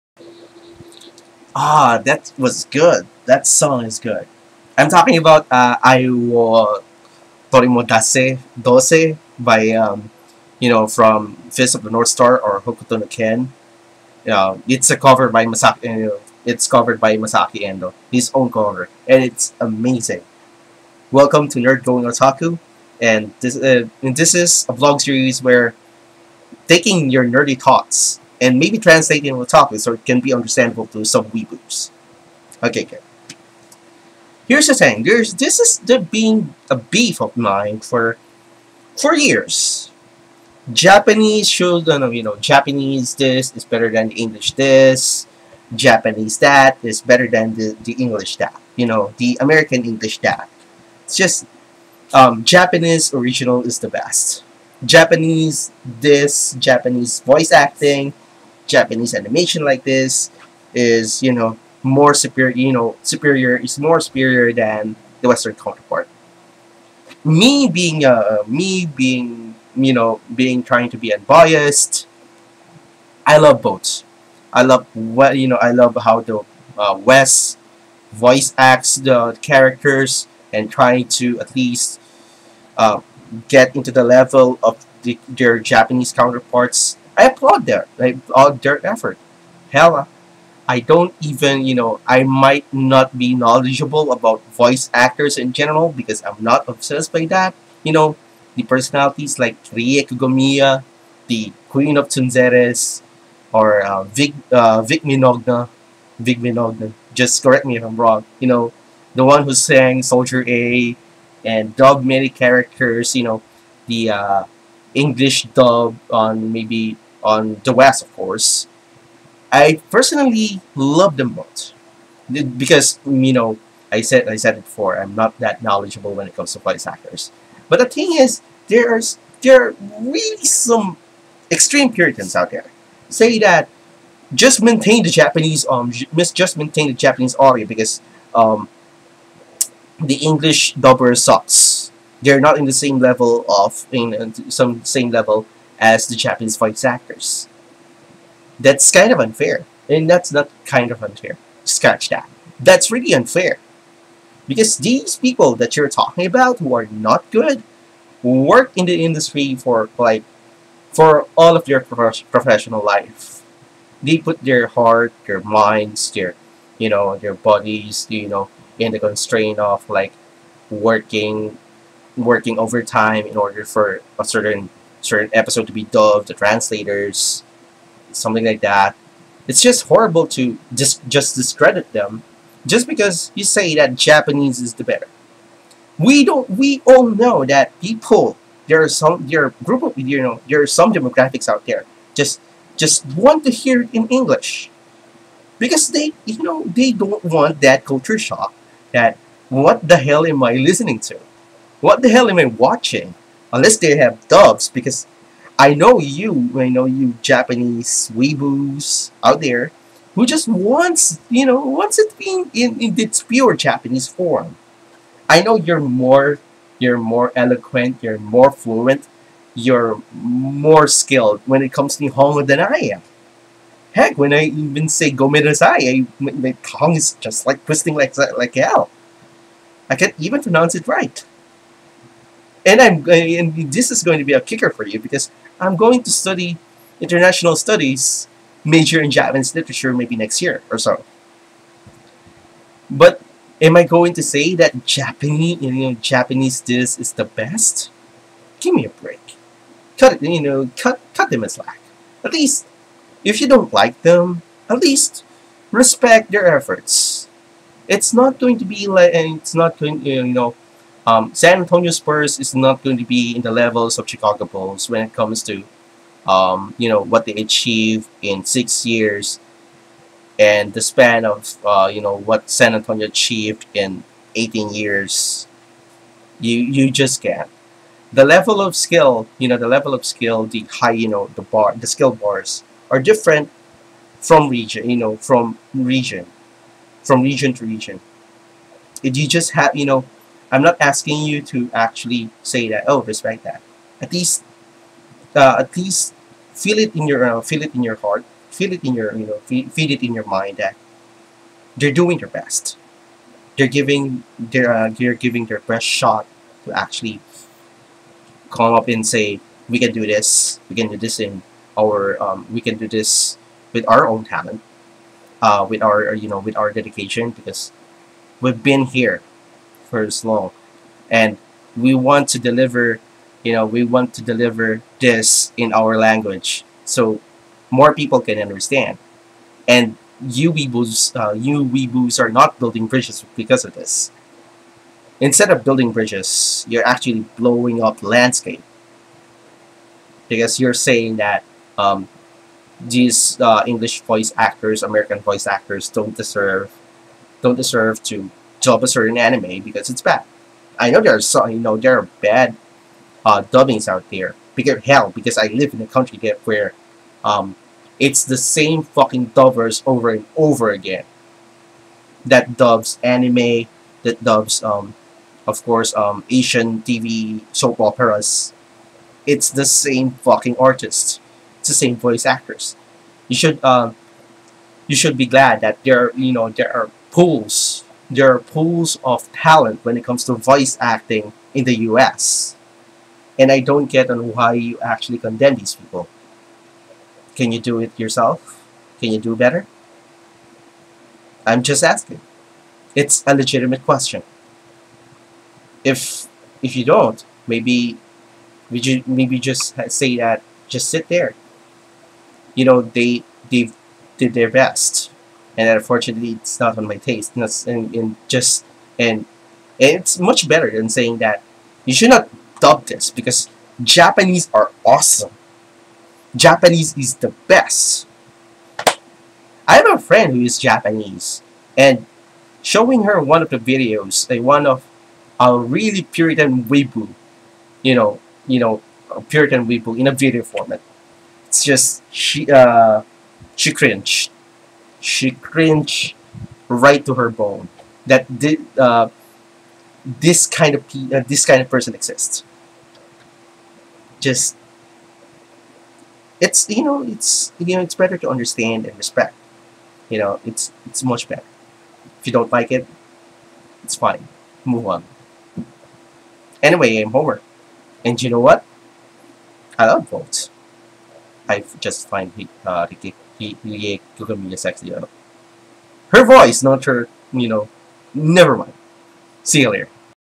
Nerd Going Otaku: Japanese voice actors are the best! English dub can go suck a dick!
It's been a while since I've uploaded one haven't I? Well, this time, let's dive into an age-old argument. Too bad these people who says that Japanese voice actors/actresses only have the right to dub anime and their English counterparts don't have the right to overdub their favorite animes is unfair on many aspects. Listen and you'll get my beef about this issue.

If you're a weaboo or a Japanese puritan and have some queries in this video, you're most welcome.

Skype ID: ramzarugliabeoulve